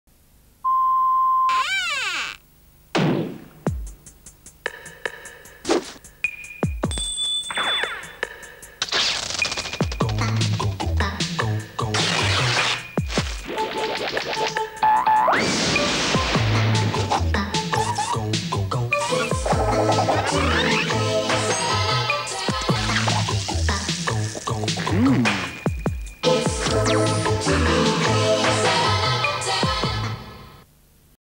Classic International 7UP Fido Dido TVC - TV Color Bar
Fido Dido is a cartoon character created by
Joanna Ferrone and Sue Rose.They first
developed the character in 1985 on a napkin in
a restaurant whilst having lunch. Fido Dido was
licensed to PepsiCo in the 1980s, with the
character becoming particularly popular during
the 1990s.